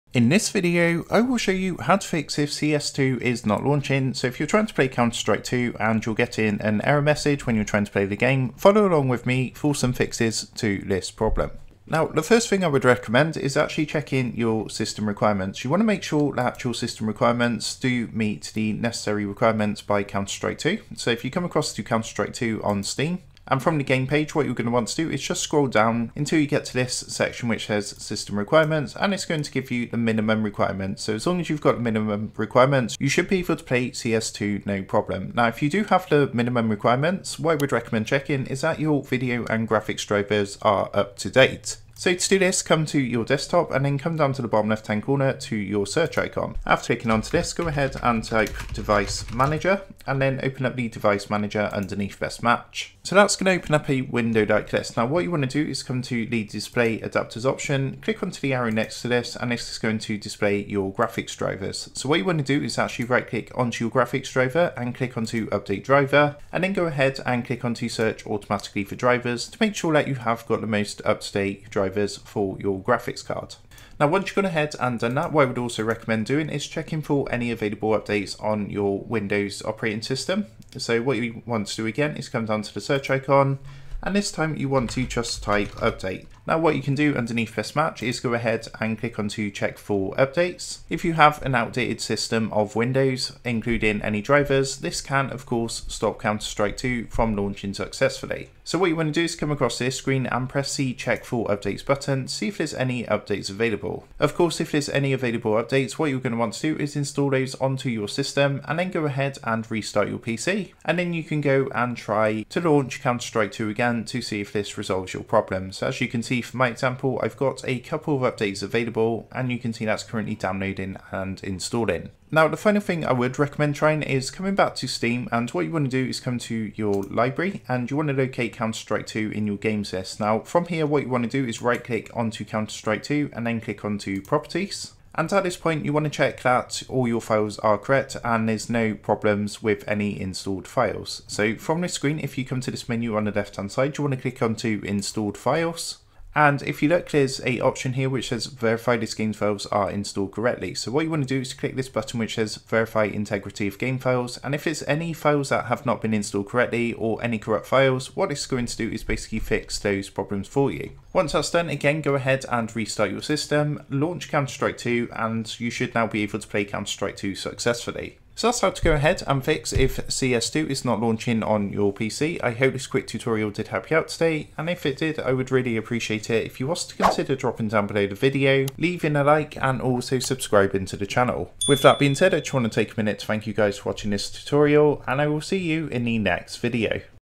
How To Fix CS2 Not Launching
Are you eager to jump into the action-packed world of Counter Strike 2, but encountering frustrating issues with the game not launching? Don't worry, we've got you covered! In this video, we'll provide you with a comprehensive troubleshooting guide to help you fix Counter Strike 2 launch issues and get back in the game.

Join us as we explore the most common reasons behind Counter Strike 2 not launching and provide you with step-by-step solutions to resolve the problem. From verifying game files to updating drivers, we'll cover all the essential fixes you need to know.

Whether you're a seasoned CS2 player or a newcomer to the game, this troubleshooting guide is tailored to help you overcome launch issues and get you back to enjoying the intense gameplay of Counter Strike 2.

Don't let technical glitches hinder your gaming experience. Watch this video now and learn how to fix Counter Strike 2 not launching like a pro!

Let's get your Counter Strike 2 back on track and dominate the battlefield!

 ✨Recommended VPN: ✨

Thanks!